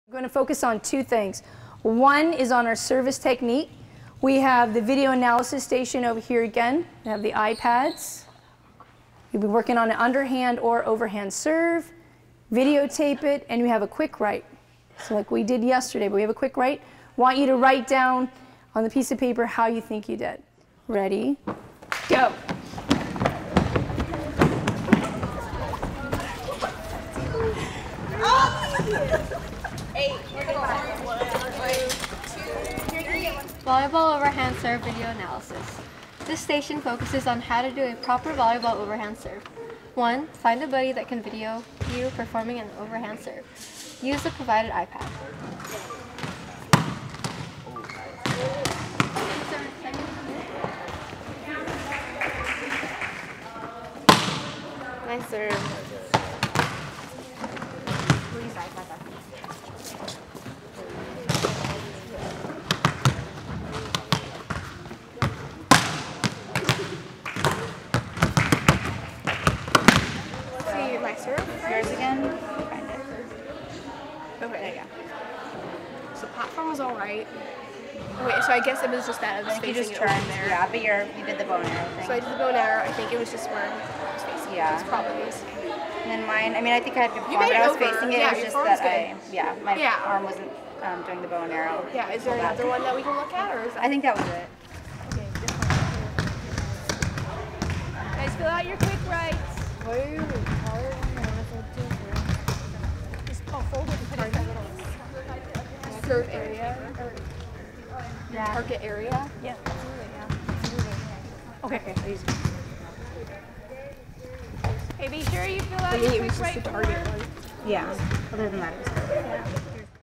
Skill Analysis With iPad
Students collaborate to use iPads to video tape serving progression, evaluate form and implement appropriate adjustments to technique.